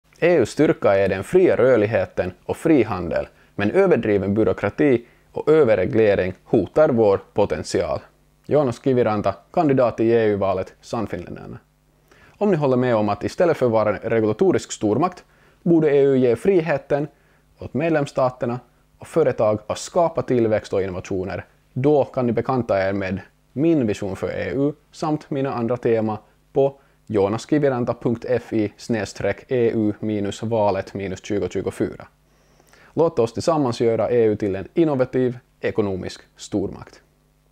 EU: från en regulatorisk stormakt till en innovativ ekonomisk stormakt
Med denna video uppmuntrar ja er att kolla min vision och tema för EU-valet 2024 på

Reklam betalas av Joonas Kiviranta.